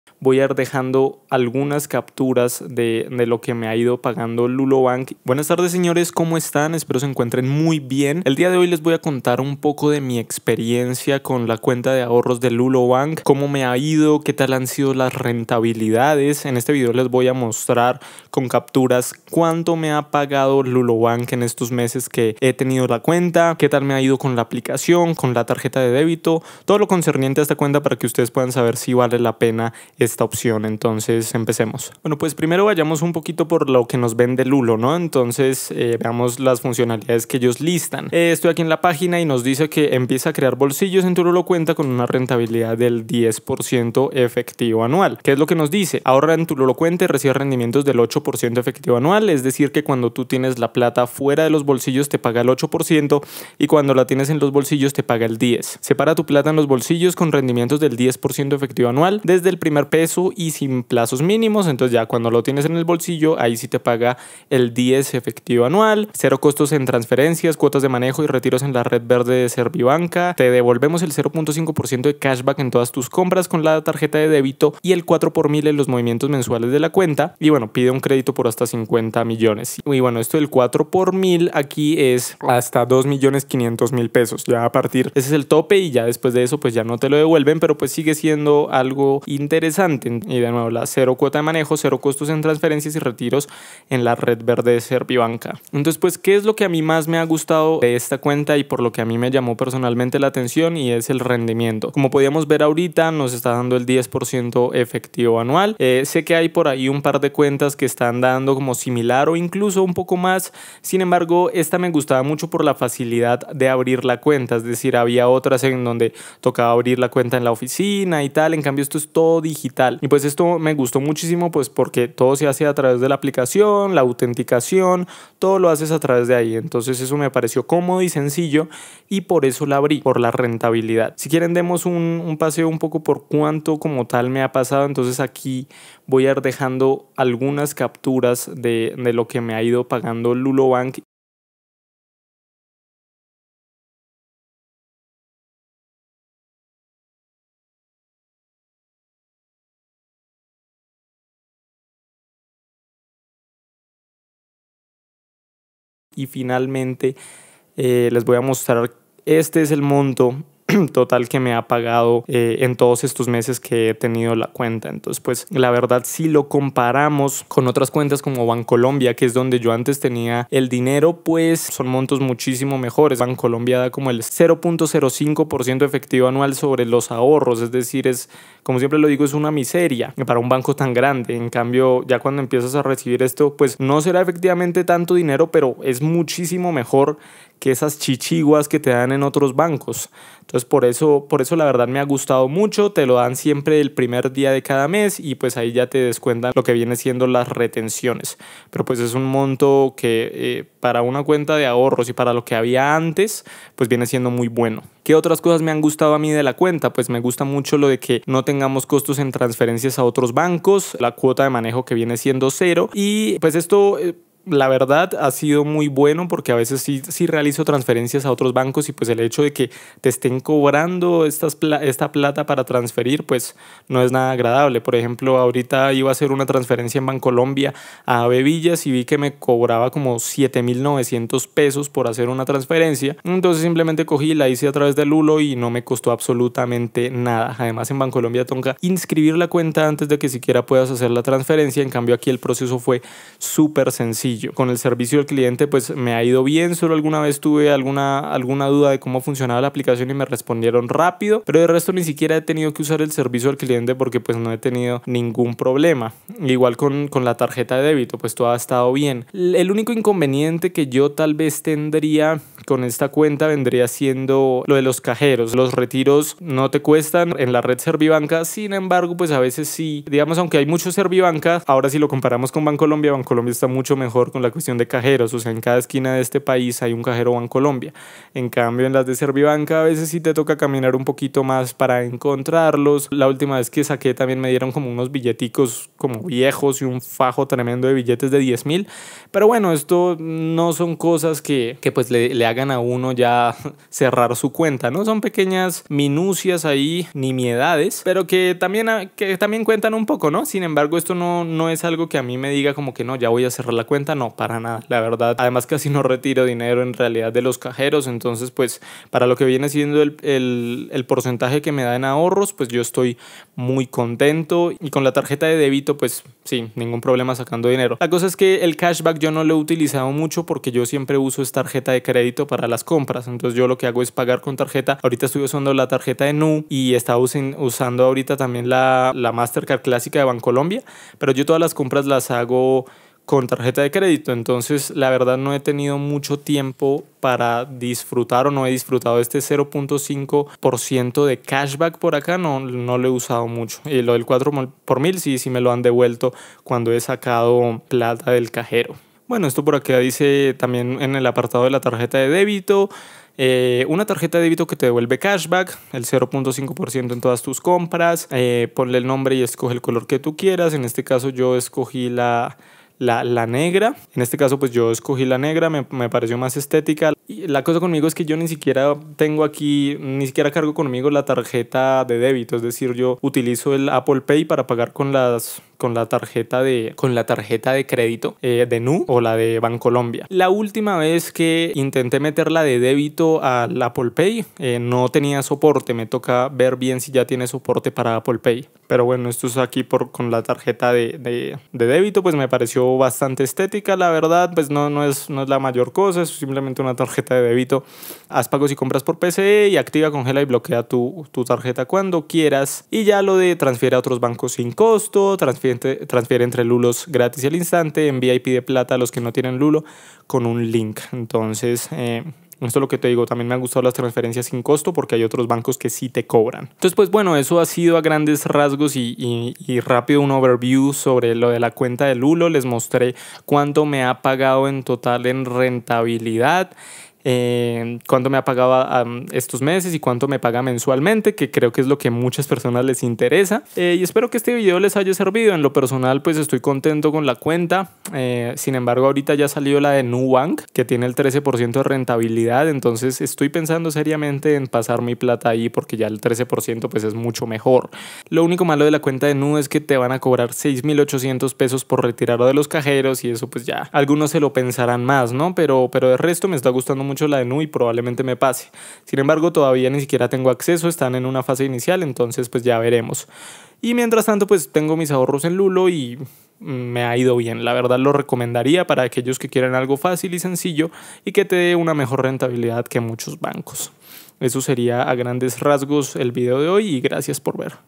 Lulo Bank Colombia: Esto me pagó en intereses después de 7 meses
En este video hablo sobre la cuenta de ahorros de Lulo Bank Colombia, las características de esta cuenta de ahorros, y cuánto me ha pagado en intereses después de 7 meses de haberla abierto.